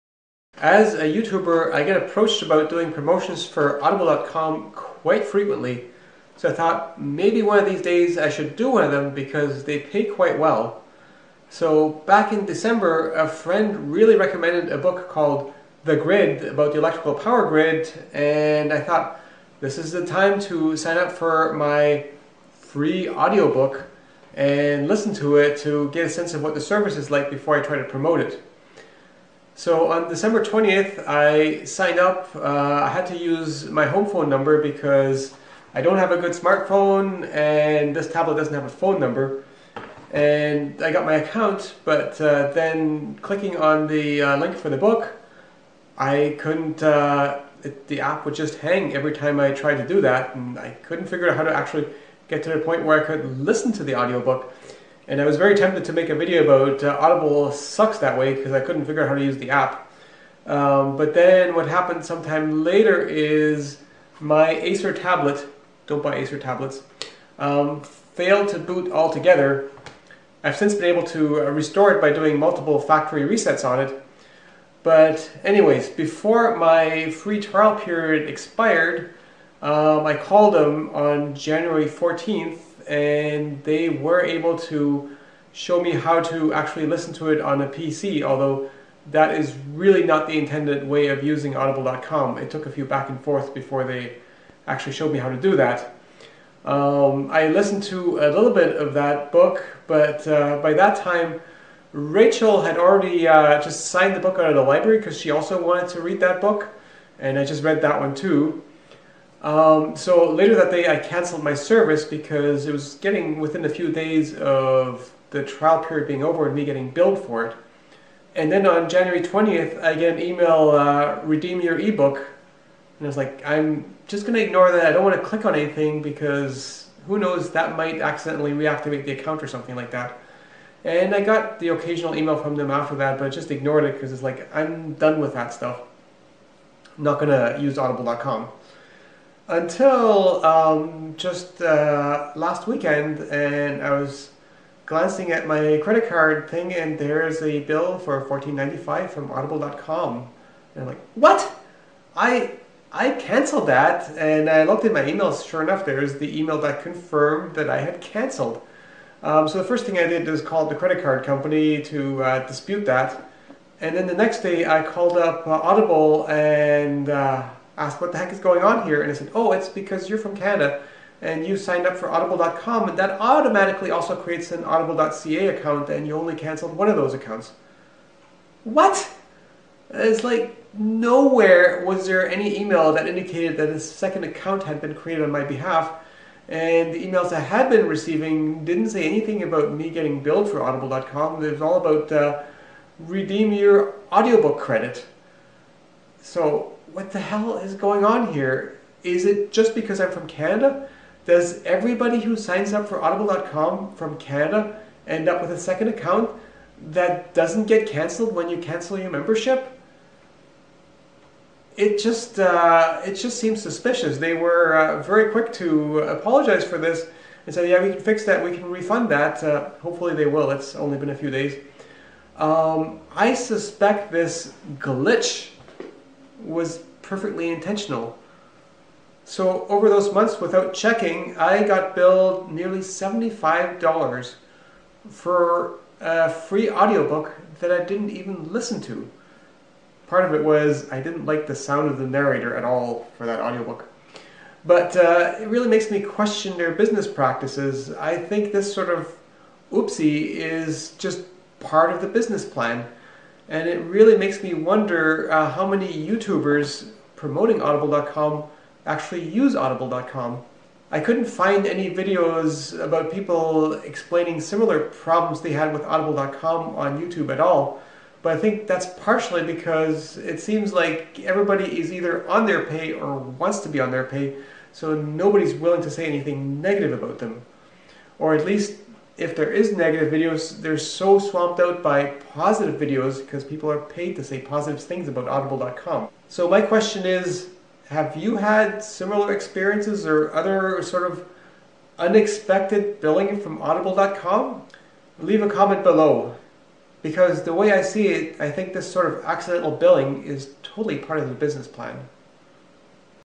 I was billed $75 for a 'free' audible.com audio book!
My audible.com experience.  I suspect this sort of "error" in their favour is totally part of the business plan